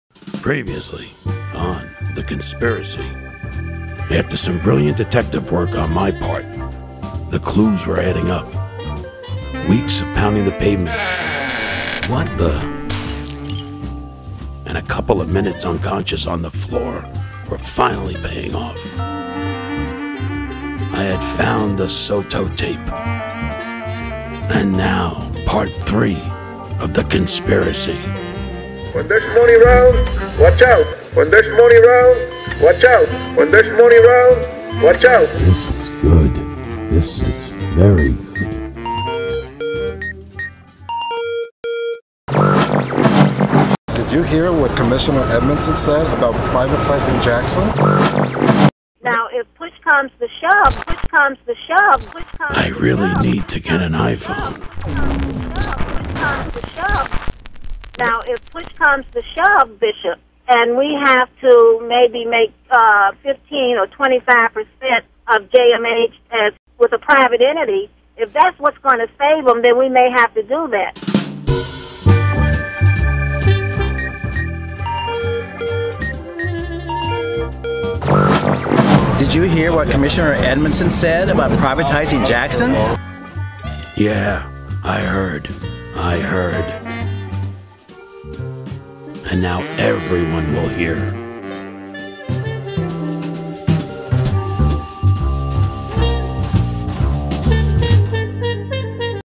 THE CONSPIRACY PT 3.mp4
Our Private Detective continues his search for clues.  Who is behind the conspiracy to rip apart Jackson Health System?

An on-going series for SEIU 1991, the Nurses and Healthcare Workers of the Jackson Health System, Miami, Florida.